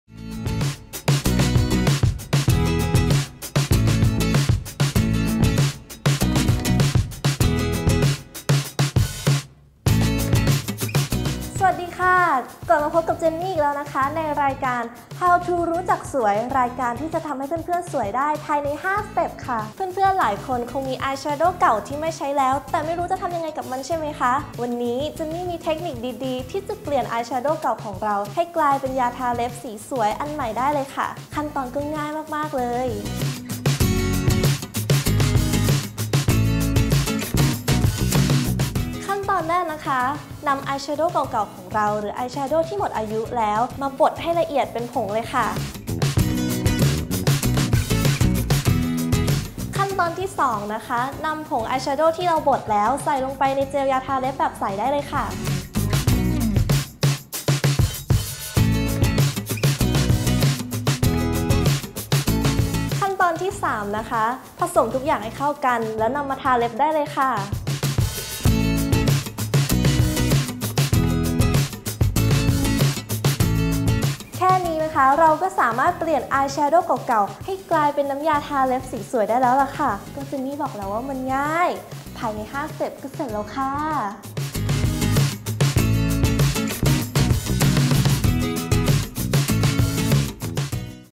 How To รู้จักสวย l เปลี่ยนอายแชโดว์เป็นยาทาเล็บขวดใหม่ สวยได้ภายใน 5 สเต็ป | Jeban
อายแชโดว์หมดอายุอย่าเพิ่งทิ้ง! เพราะรู้มั้ยว่า สามารถเอามาเปลี่ยนเป็นยาทาเล็บขวดใหม่ได้!